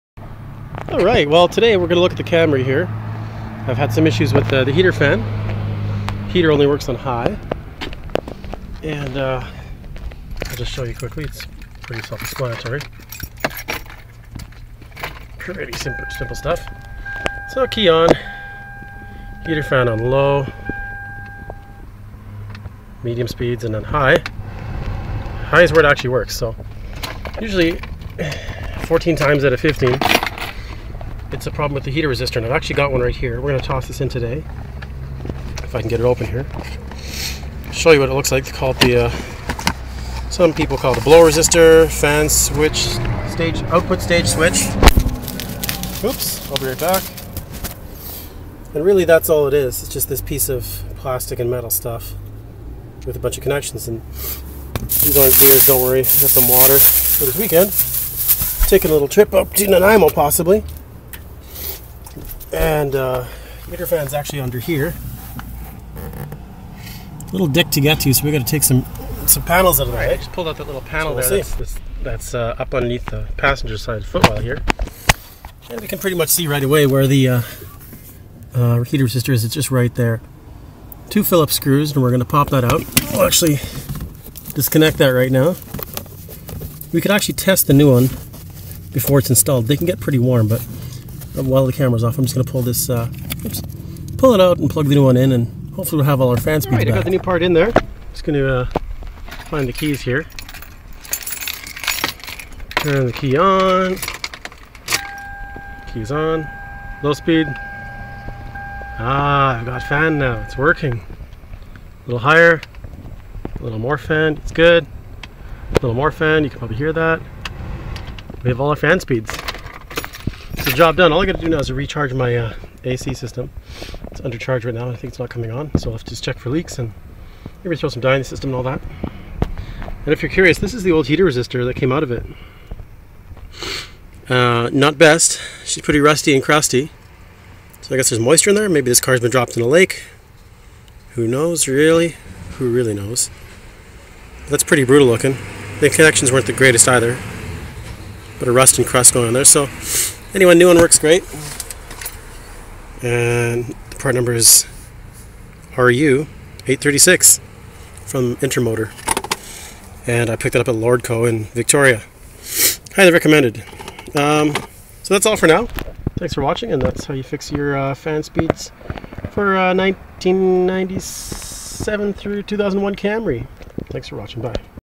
2000 Toyota Camry blower resistor replacement
Fan speed issues?
Only working on high speed?
This is likely your issue.
I'm sure this is the same on '97 to '01 Camrys... unless it has some funky auto AC option, then I'm not sure...
Pretty easy repair, part cost me like $24
Cheap and easy fix.

Way she goes.